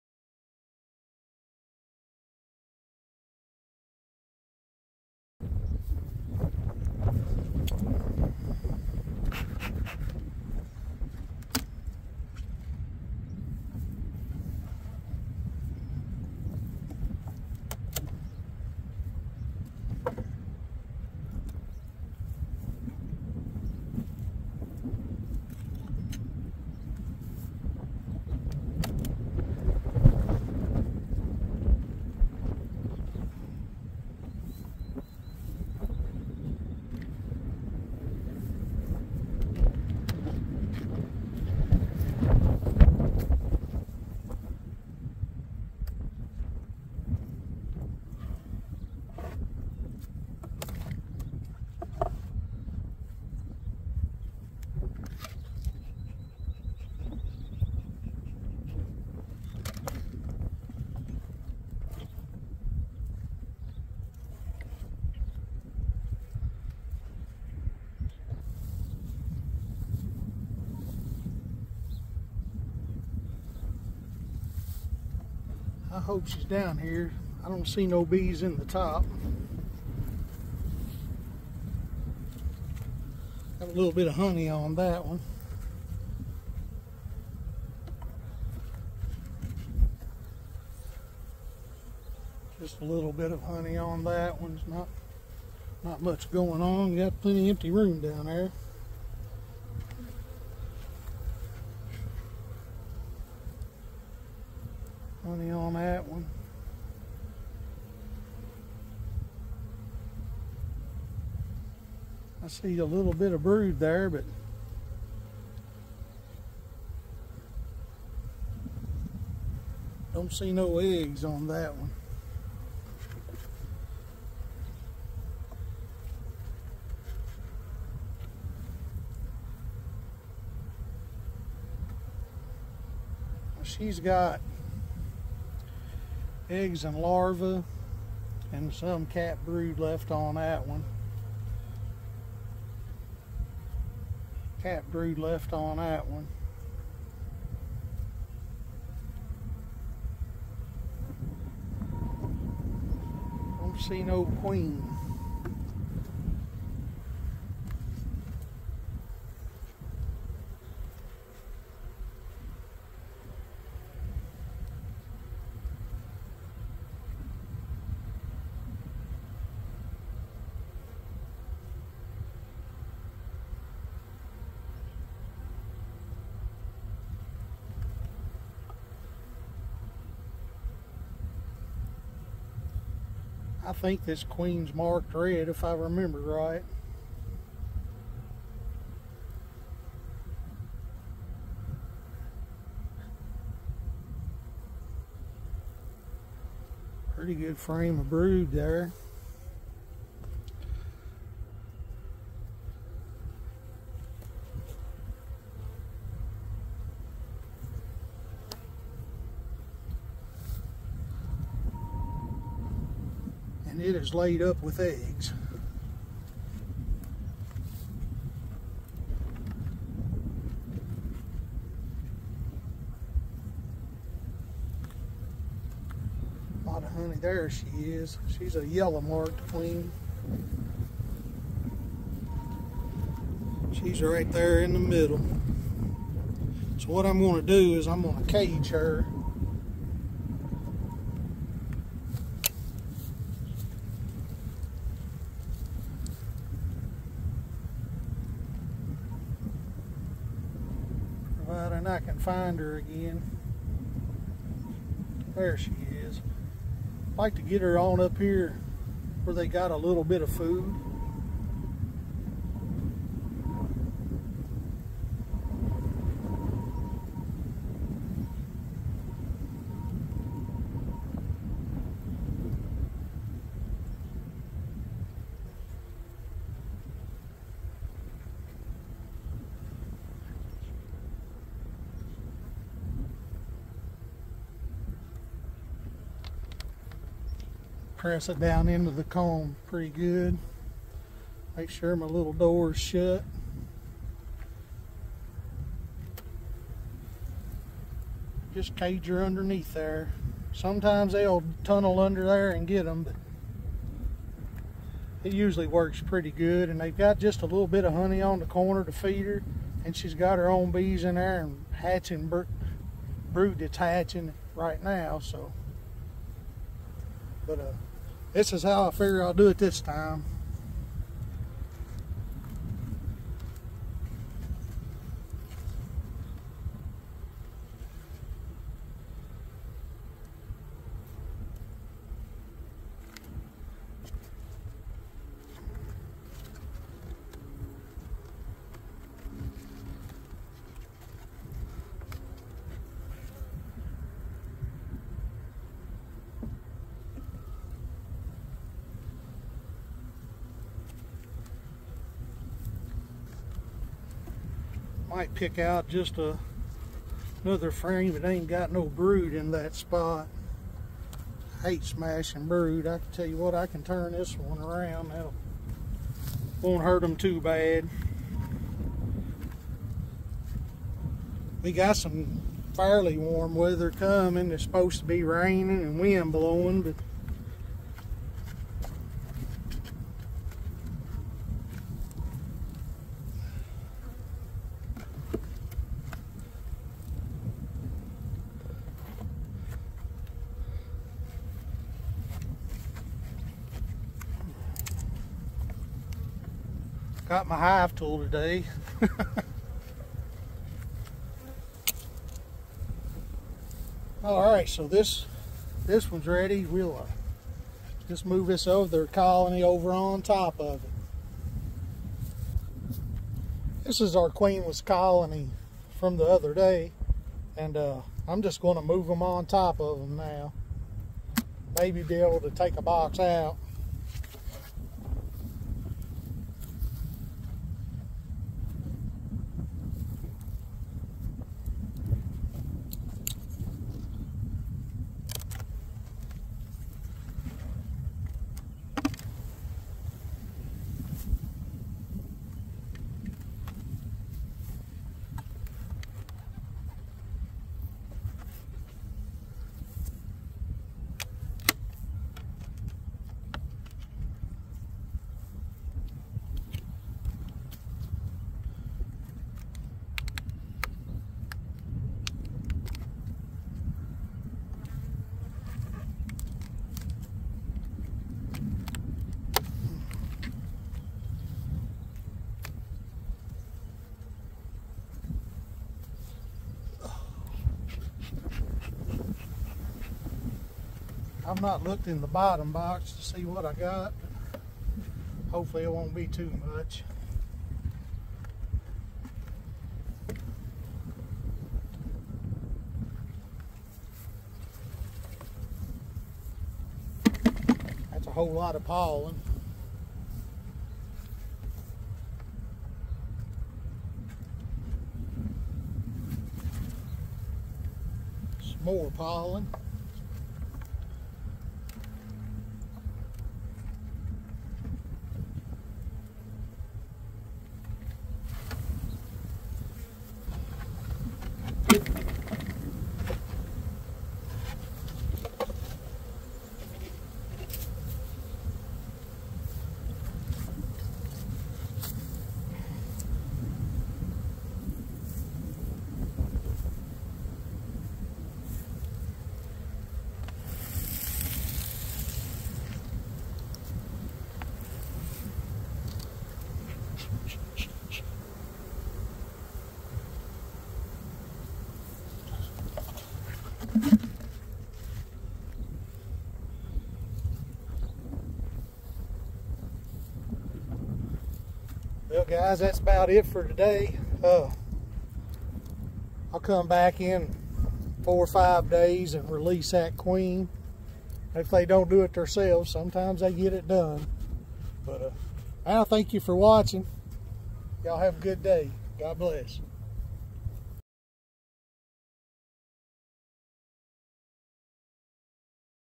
beekeeping : caging queen combining two colonies/ # 27
combinding the colony i made queenless